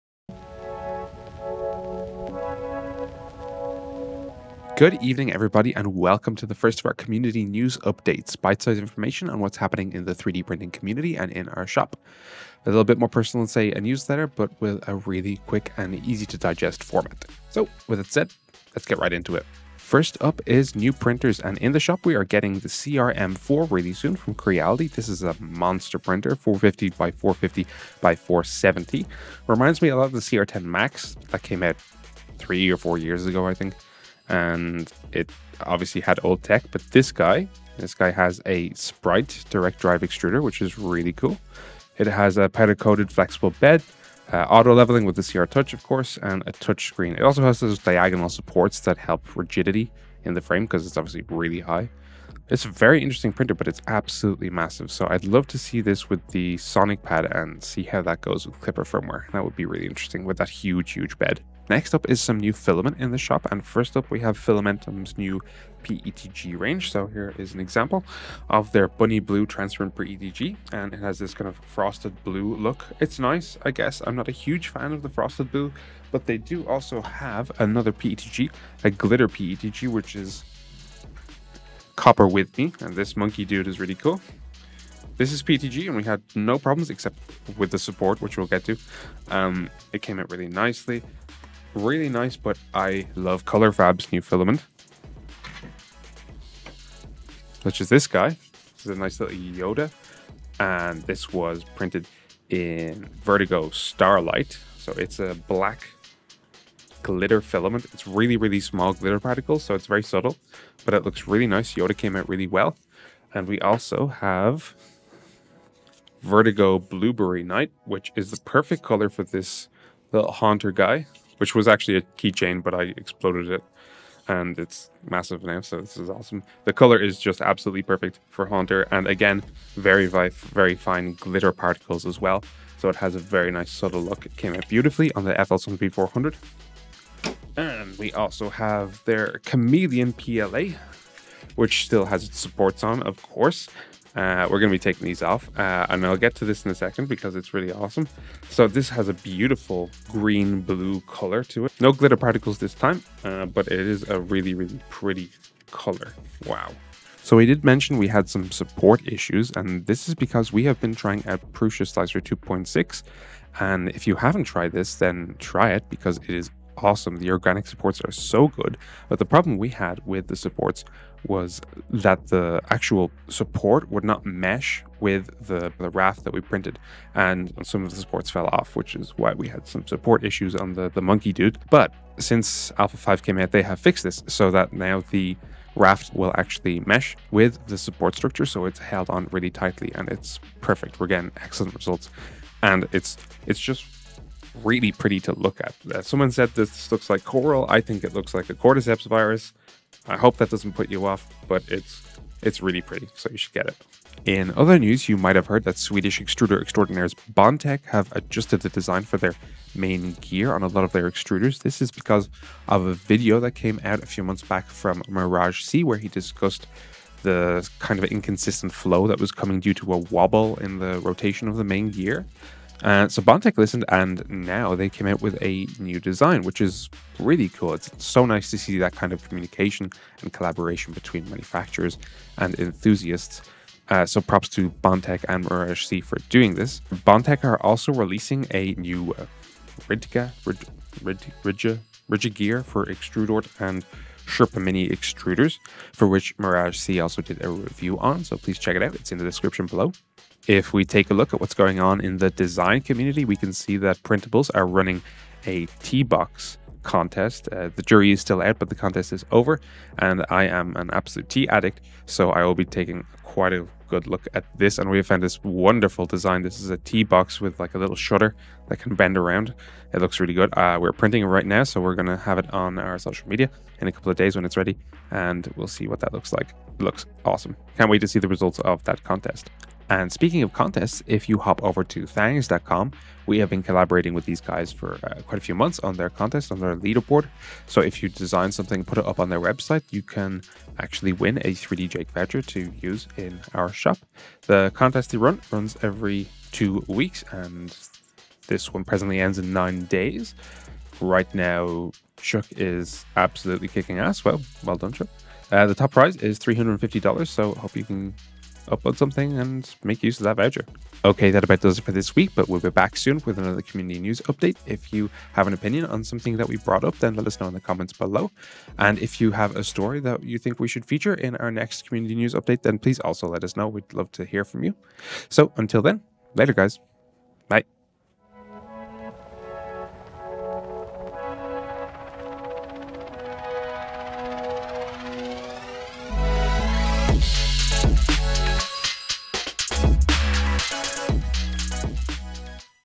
3D Printing Community News #1
Welcome to our first Community News! We'll be bringing you quick and frequent updates on what is going on in the 3D Printing Community!

⭐Thangs Contest! Win a 3DJake Voucher⭐